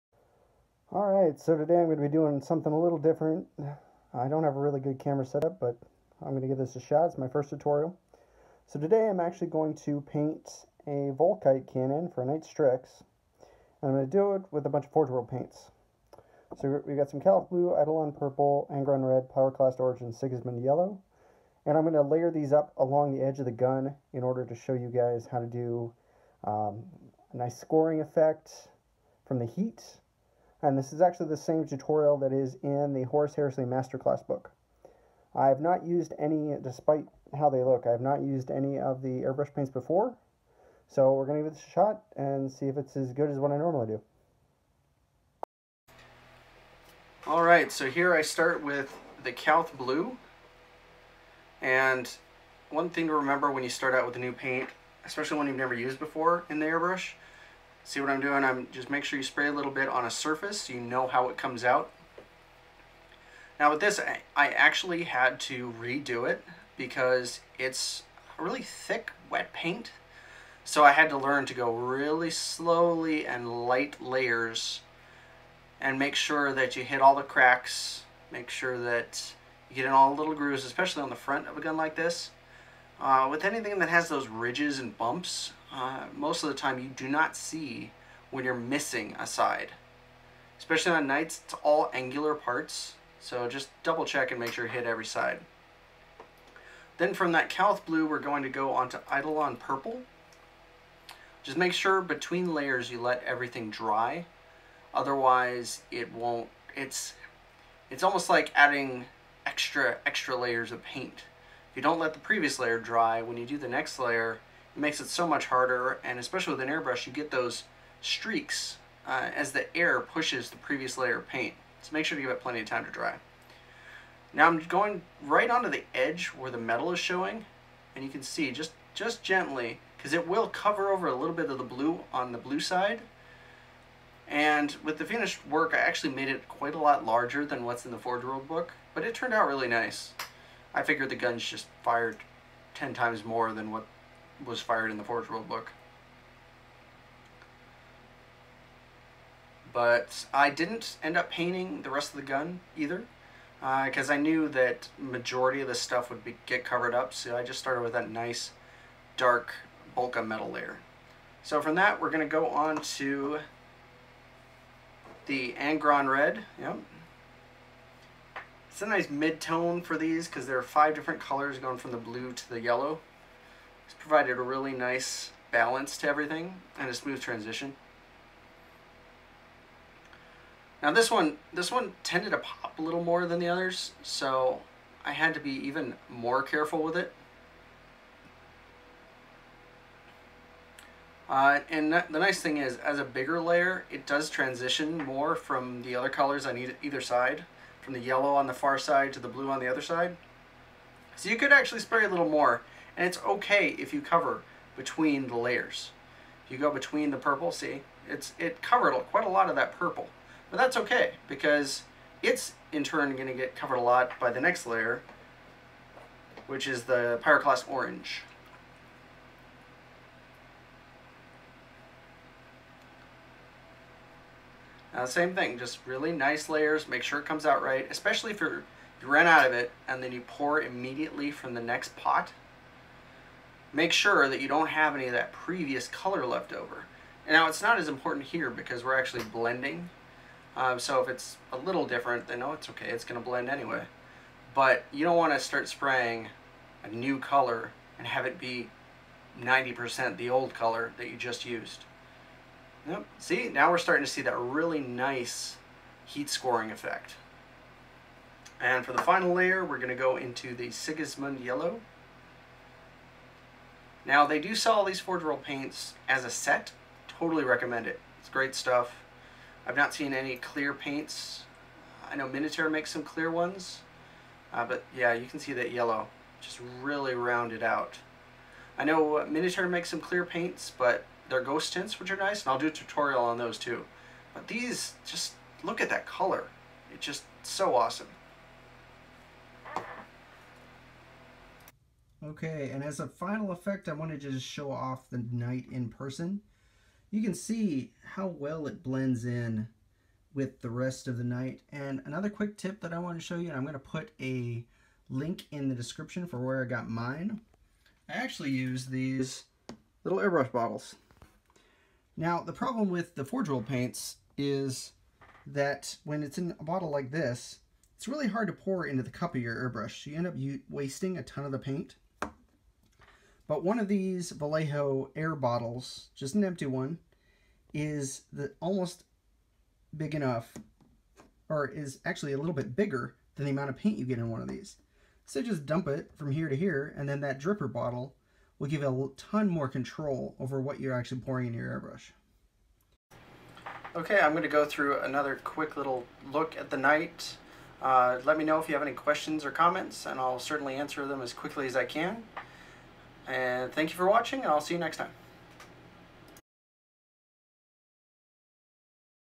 Forgeworld Clear Airbrush Paint Knight Stryix Heat Scoring Tutorial
All channel content is made possible through the generous support of my Patrons.

In this tutorial I will show you how to use forgeworld airbrush paints to apply heat scoring to the Volkite cannon of a knight stryix.